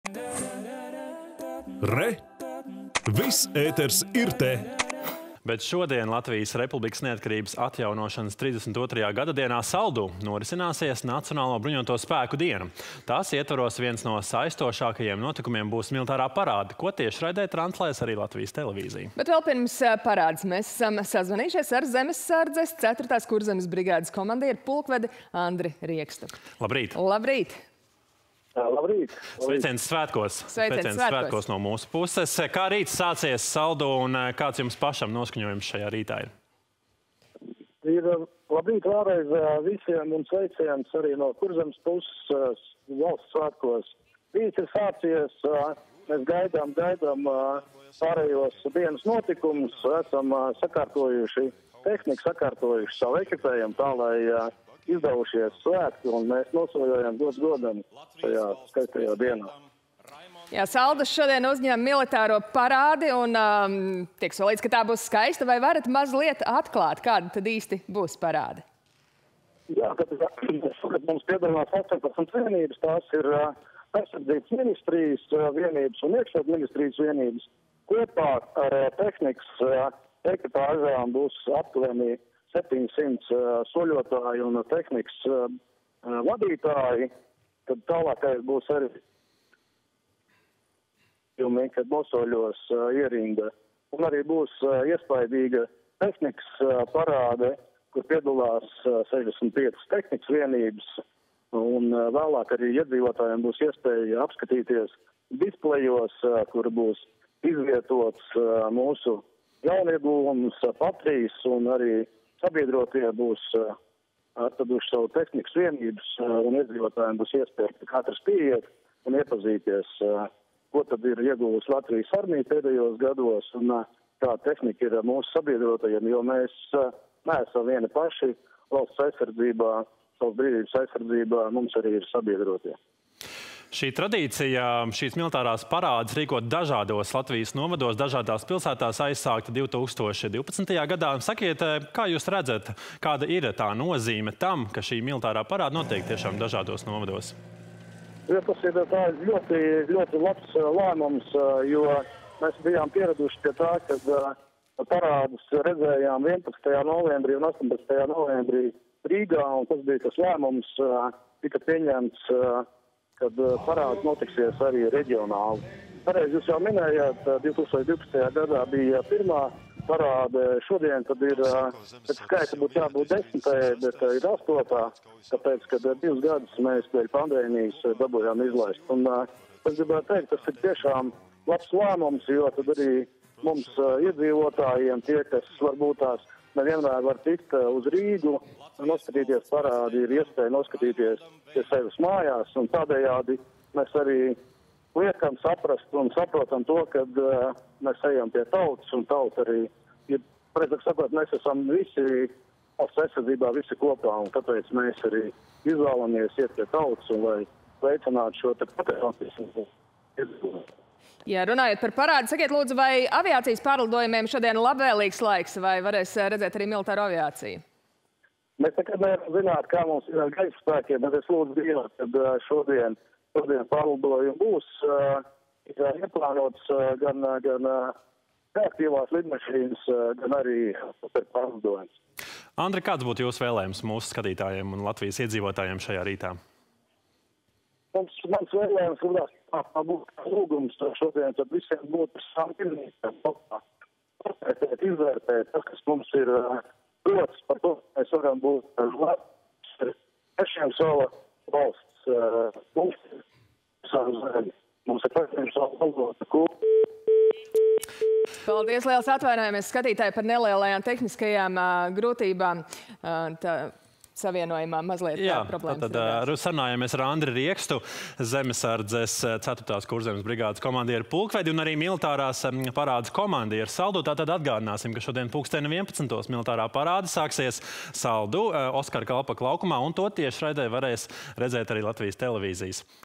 Telefonintervija ar Zemessardzes 4. Kurzemes brigādes komandieris pulkvedi Andri Riekstu
Šodien – Latvijas Republikas neatkarības atjaunošanas 32. gadadienā – Saldū norisināsies Nacionālo bruņoto spēku diena. Tās ietvaros viens no saistošākajiem notikumiem būs militārā parāde, ko tiešraidē translēs Latvijas Televīzija.
Bet vēl pirms parādes esam sazvanījušies ar Zemessardzes 4. Kurzemes brigādes komandieri, pulkvedi Andri Riekstu